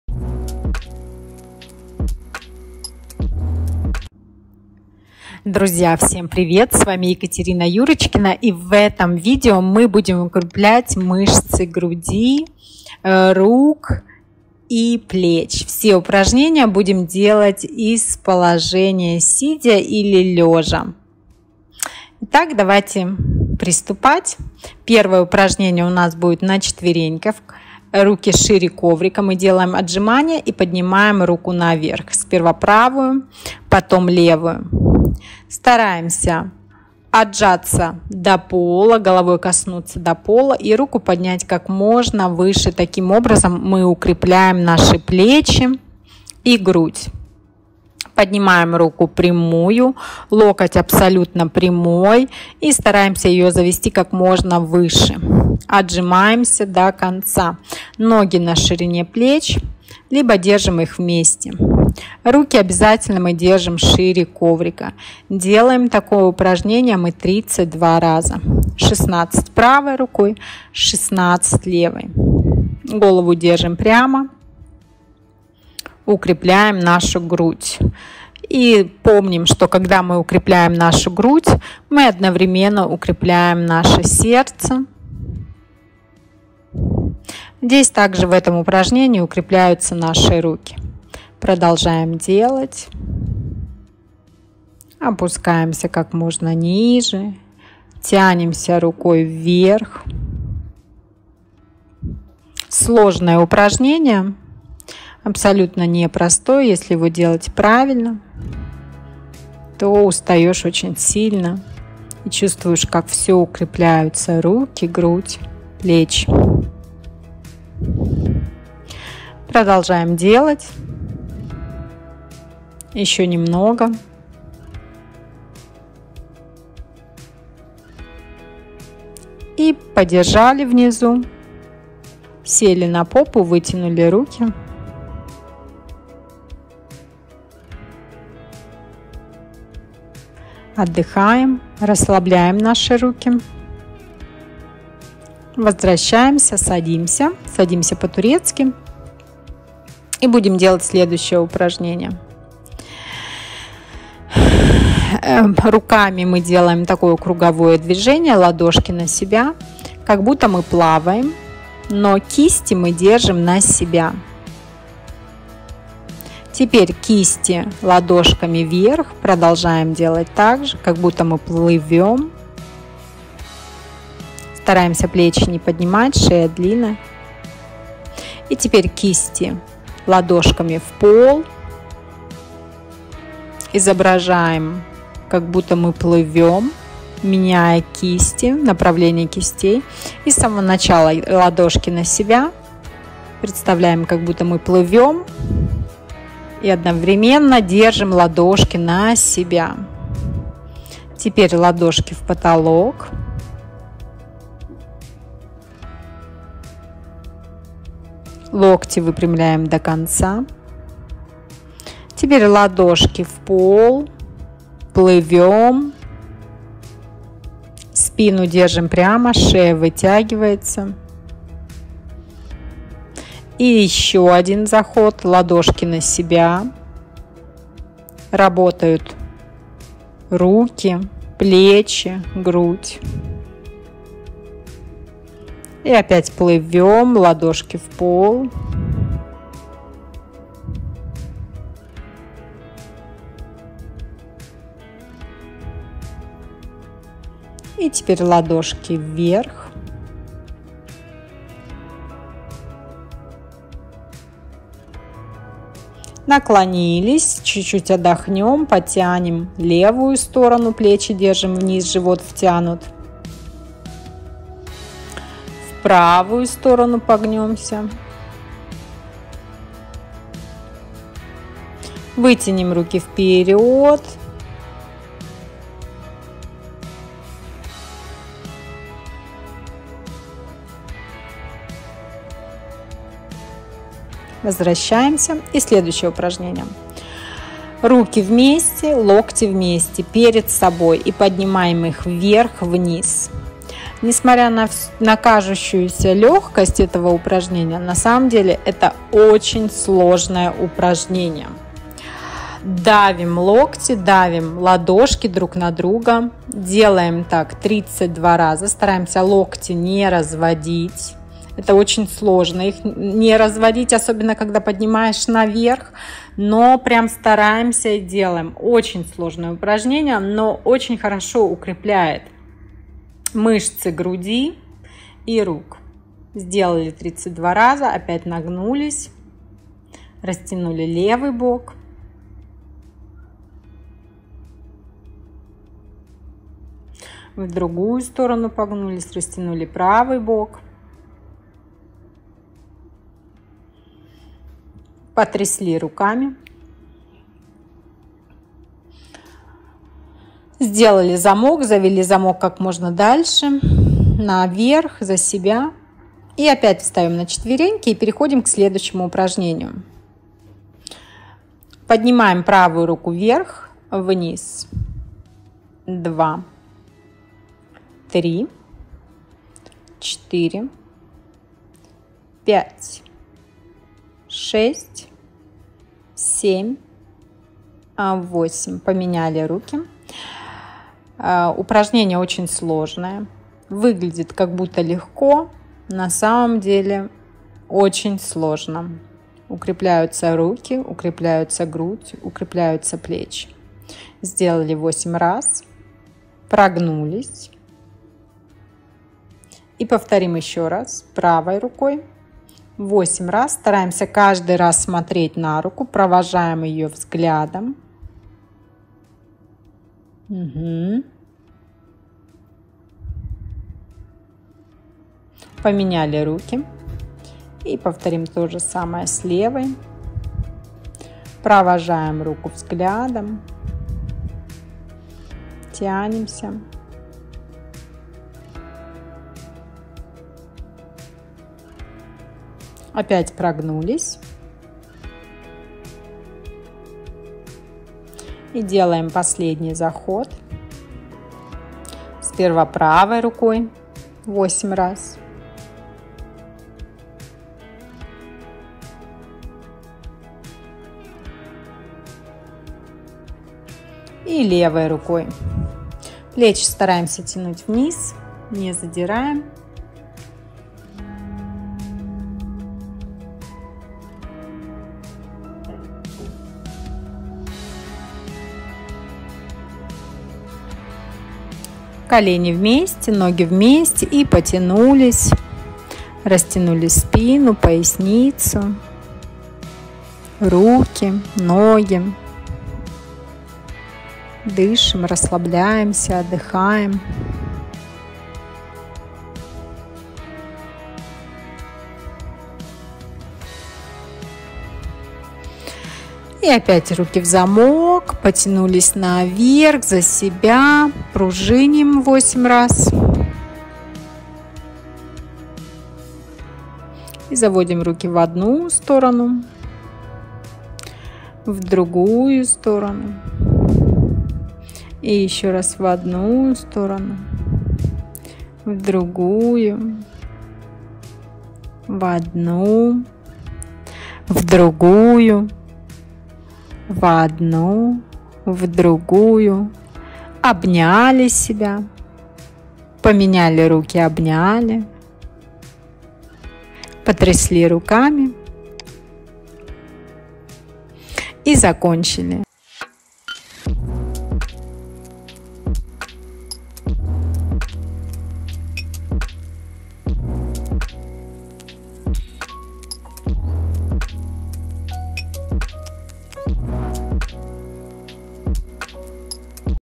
Как сделать красивыми руки, плечи и грудь
В этом видео я Вам покажу комплекс упражнений, который поможет проработать мышцы грудного отдела, плеч и рук. Достаточно уделить всего 10 минут в день, чтобы вы уже увидели первые результаты. Будьте красивы и здоровы.